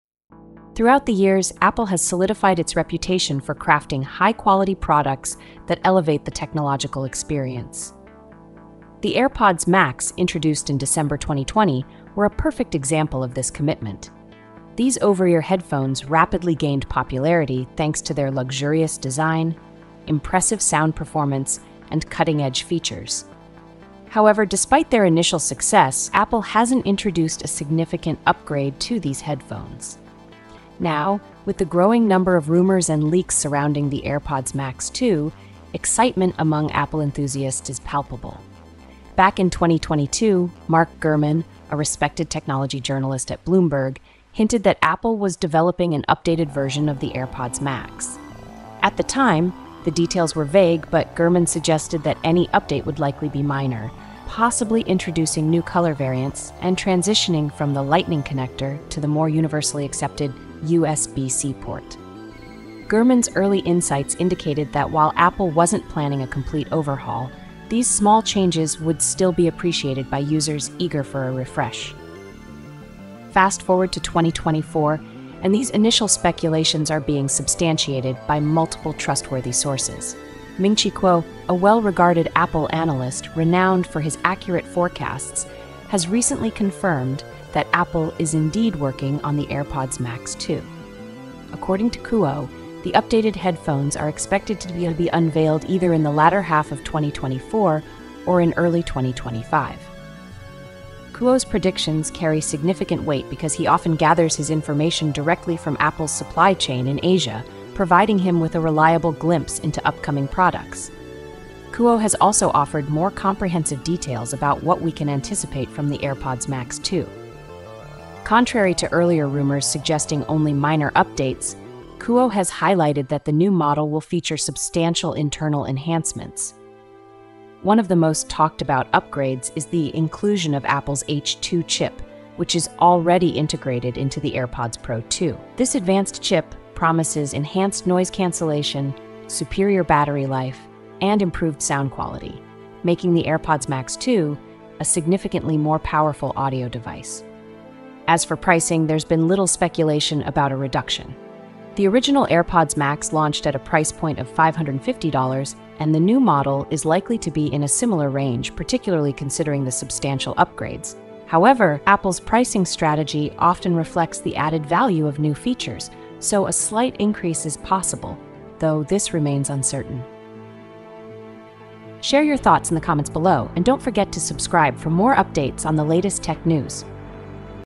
Airpods Max 2 - Price & Release Date or Launching Soon?!
Welcome to our latest video where we dive into everything you need to know about the highly anticipated AirPods Max 2! 📱🎧

In this video, we cover:

🔍 Release Date Predictions: Get the scoop on when you can expect the AirPods Max 2 to hit the market, with insights from top tech analysts like Ming-Chi Kuo and Mark Gurman.

💰 Price Expectations: Discover the potential pricing for the AirPods Max 2 and whether we might see any changes compared to the original AirPods Max.

✨ New Features & Upgrades: Explore the exciting new features coming to the AirPods Max 2, including the revolutionary H2 chip, improved noise cancellation, and the switch from Lightning to USB-C.

🎨 Design Refresh: Find out about the possible new color options and design tweaks that could make the AirPods Max 2 even more appealing.

📍 Enhanced Tracking: Learn about the inclusion of the U1 chip for precise location tracking within Apple’s Find My network.

We dive deep into these updates and provide expert analysis on what to expect from Apple’s next big audio product. Whether you’re an Apple enthusiast or just curious about the latest tech, this video is a must-watch!